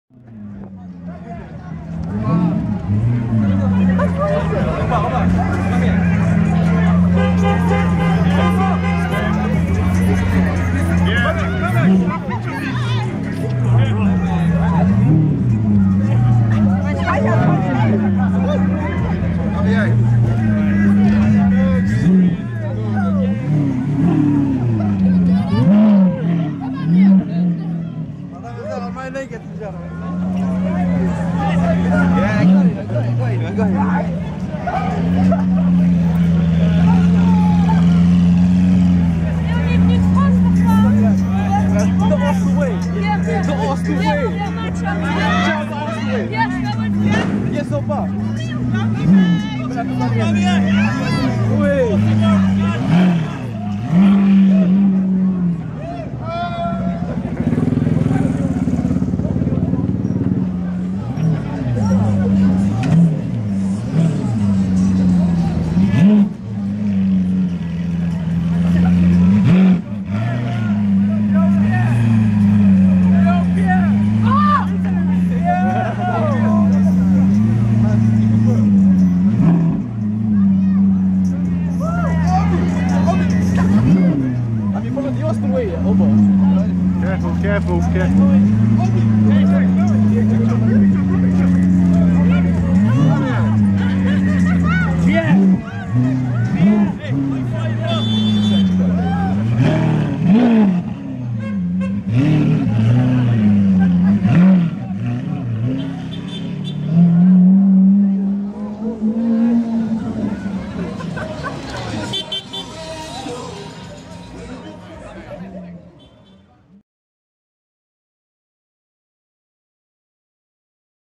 Aubameyang, his Lamborghini and a Squashed Foot
Pierre-Emerick Aubameyang leaves Emirates Stadium in his car and gets caught up in road AND fan traffic on Sunday 22nd April 2018.  At 1 min 13 secs a boy's foot finds it way under the rear wheel.  Arsenal had beaten West Ham United 4-1.